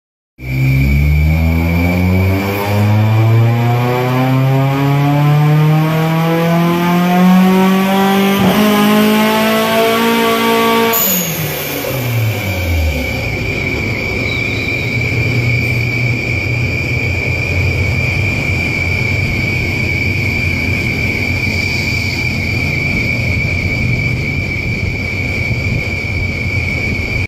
SpeedLab Rotrex Supercharged Civic FD 2.0L K20Z2 205whp
Another world's first! (Seems to be a lot lately) SpeedLab Rotrex Supercharged Honda Civic K20Z2 makes 205whp! Up from 125whp stock!